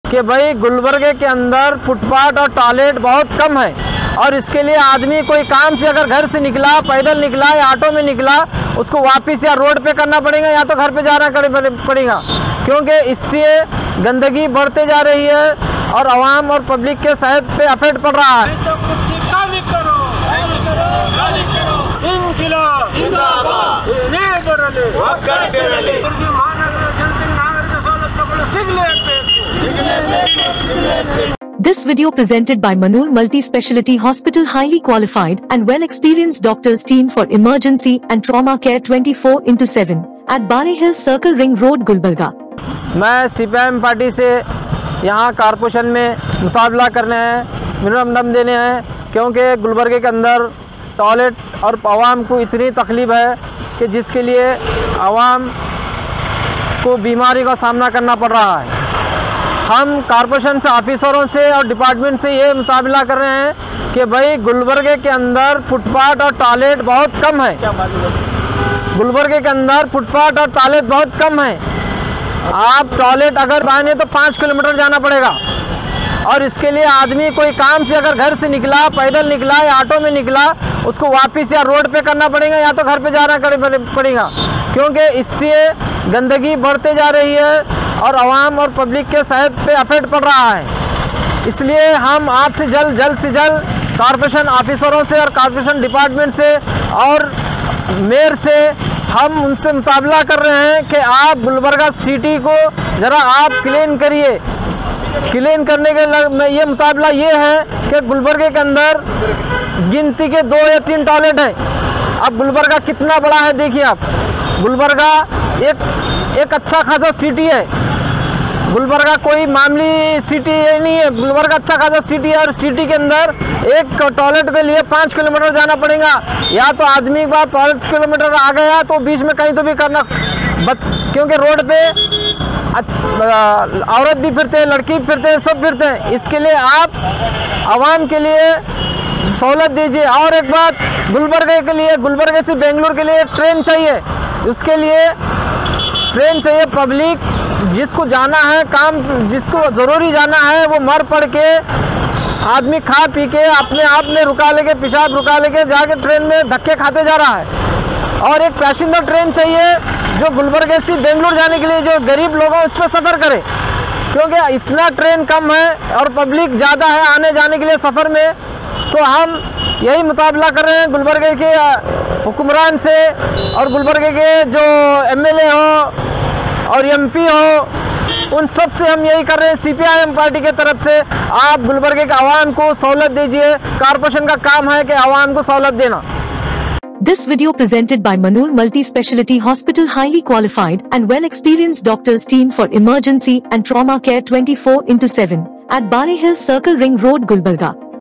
CPI(M) Protesters Demand Increased Trains Between Gulbarga and Bangalore | Highlight Various Issues
CPI(M) Protesters Demand Increased Trains Between Gulbarga and Bangalore, and Highlight Various Issues